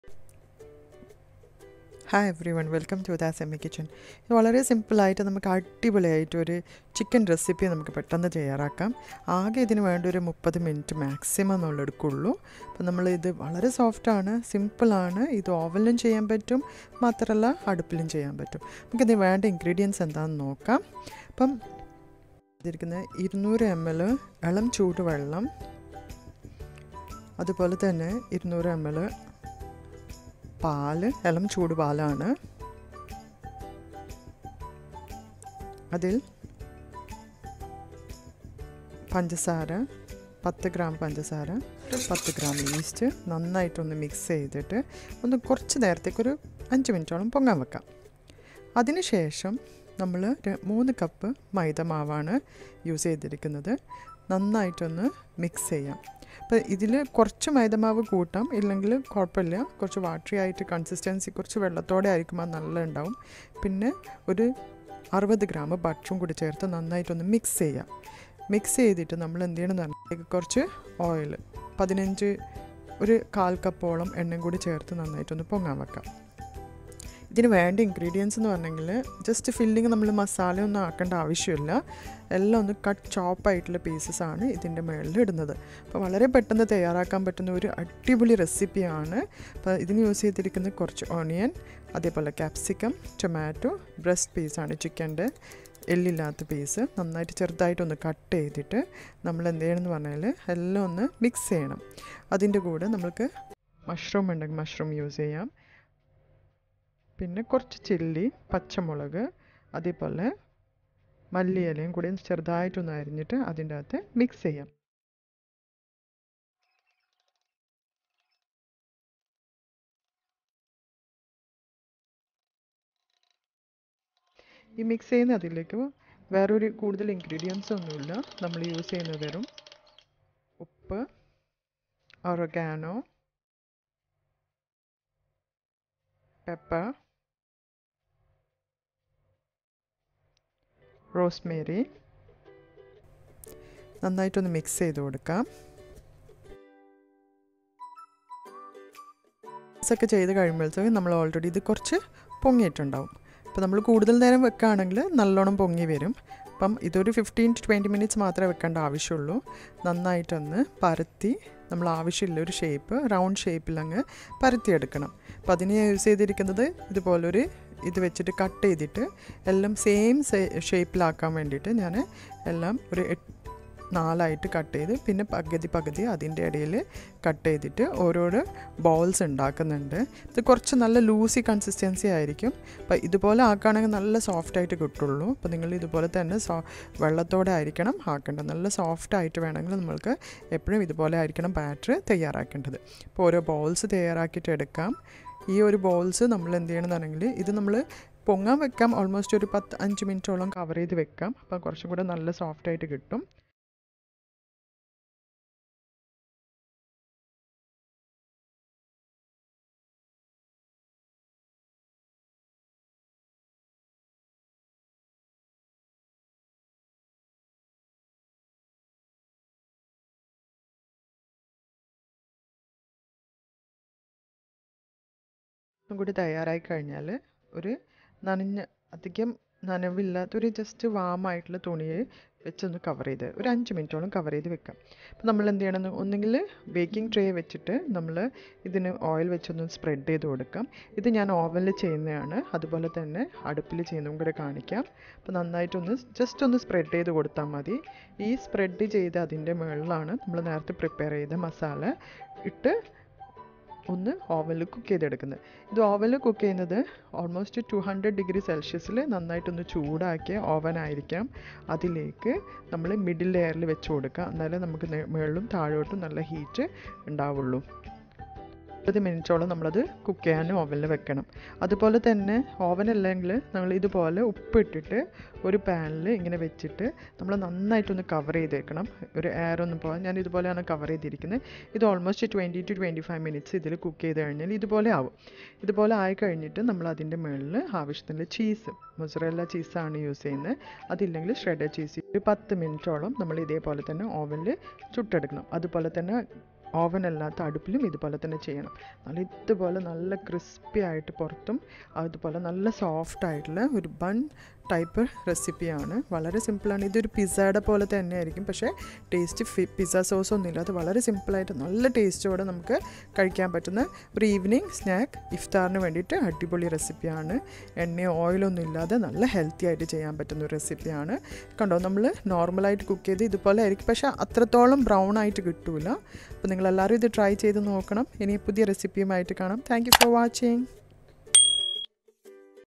Iftar recipe|No fry,with and without oven|Chicken recipe|നോമ്പ് തുറക്കുമ്പോൾ ഇതിൽ ഒരെണ്ണം മതി [NEW]
Do you have chicken breast at home with no time prepare this delicious snack recipe for Iftar special...

A small mistake while giving voice.. pls follow this for correct measurements 📌📌📌

Ingredients
For dough
 salt
3 Cups flour
oil - 15ml
butter -60g

filling
Chicken breast
 Onions
tomatoes
Capsicum

Coriander leaves
Mushrooms
Spring onion

 salt

 black pepper
oregano
rosemary

200 grams of grated Mozzarella cheese

Delicious Recipes

Please do watch for detailed  video

@udasyummykitchen Instagram